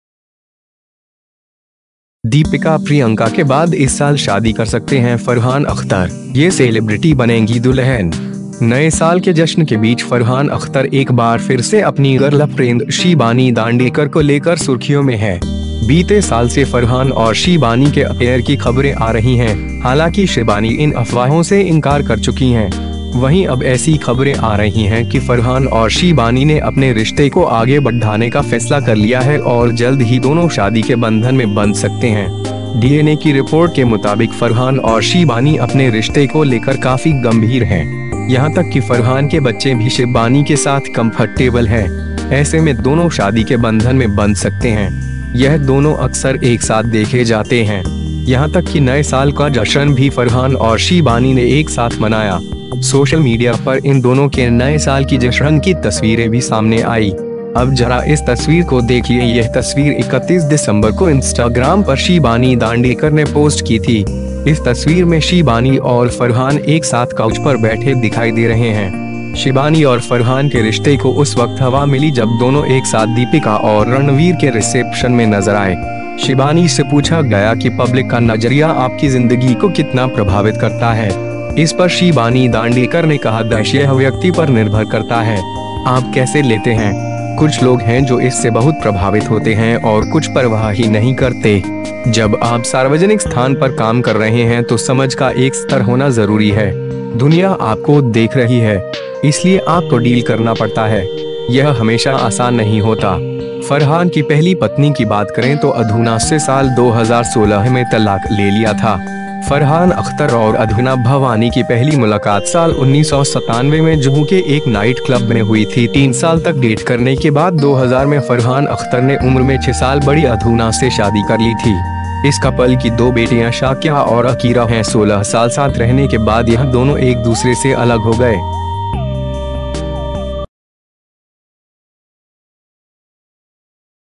दीपिका प्रियंका के बाद इस साल शादी कर सकते हैं फरहान अख्तर, ये सेलिब्रिटी बनेंगी दुल्हन
नए साल के जश्न के बीच फरहान अख्तर एक बार फिर से अपनी गर्लफ्रेंड शिबानी दांडेकर को लेकर सुर्खियों में है। बीते साल से फरहान और शिबानी के अफेयर की खबरें आ रही हैं, हालांकि शिबानी इन अफवाहों से इंकार कर चुकी हैं। वहीं अब ऐसी खबरें आ रही हैं कि फरहान और शिबानी ने अपने रिश्ते को आगे बढा़ने का फैसला कर लिया है और जल्द ही दोनों शादी के बंधन में बंध सकते हैं।

डीएनए की रिपोर्ट के मुताबिक फरहान और शिबानी अपने रिश्ते को लेकर काफी गंभीर हैं। यहां तक कि फरहान के बच्चे भी शिबानी के साथ कंफटेबल हैं। ऐसे में दोनों शादी के बंधन में बंध सकते हैं। यह दोनों अक्सर एक साथ देखे जाते हैं। यहां तक कि नए साल का जश्न भी फरहान और शिबानी ने एक साथ मनाया। सोशल मीडिया पर इन दोनों के नए साल की जश्न की तस्वीरें भी सामने आई।

अब जरा इस तस्वीर को देखिए यह तस्वीर 31 दिसंबर को इंस्टाग्राम पर शिबानी दांडेकर ने पोस्ट की थी। इस तस्वीर में शिबानी और फरहान एक साथ काउच पर बैठे दिखाई दे रहे हैं। शिबानी और फरहान के रिश्ते को उस वक्त हवा मिली जब दोनों एक साथ दीपिका और रणवीर के रिसेप्शन में नजर आए।

शिबानी से पूछा गया कि पब्लिक का नजरिया आपकी जिंदगी को कितना प्रभावित करता है। इस पर शिबानी दांडेकर ने कहा - 'यह व्यक्ति पर निर्भर करता है। आप कैसे लेते हैं। कुछ लोग हैं जो इससे बहुत प्रभावित होते हैं और कुछ परवाह ही नहीं करते। जब आप सार्वजनिक स्थान पर काम कर रहे हैं तो समझ का एक स्तर होना जरूरी है। दुनिया आपको देख रही है। इसलिए आपको डील करना पड़ता है। यह हमेशा आसान नहीं होता।'

फरहान की पहली पत्नी की बात करें तो अधूना से साल 2016 में तलाक ले लिया था। फरहान अख्तर और अधूना भवानी की पहली मुलाकात साल 1997 में जुहू के एक नाइट क्लब में हुई थी। 3 साल तक डेट करने के बाद 2000 में फरहान अख्तर ने उम्र में 6 साल बड़ी अधूना से शादी कर ली थी। इस कपल की दो बेटियां-शाक्या और अकीरा हैं। 16 साल साथ रहने के बाद यह दोनों एक दूसरे से अलग हो गए।